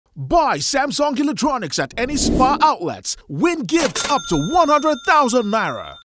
SPAR MARKET SHOPPING SPREE WITH SAMSUNG
Shop & Win today with Samsung at Spar! Buy any Samsung electronics or appliance at Spar outlets Nationwide and Win AMAZING Gifts worth up to ₦100,000.

Terms and Condition Apply.